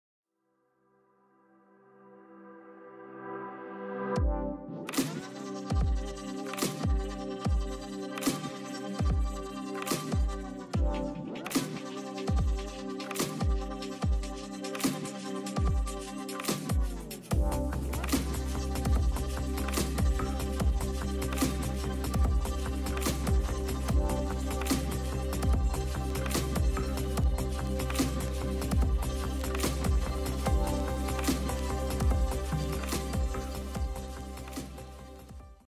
gouache painting Time-lapse | Tun Razak Exchange (TRX) |microart
💙A piece inspired from another retreat project with @haywherenext . This painting is based on our stay at @intanpayung.my , a lovely and spacious villa surrounded by nature. A great place for families to get together and reconnect with your loved ones.

Gouache on Dark Grey Pastel Paper
Size 4cm x 4cm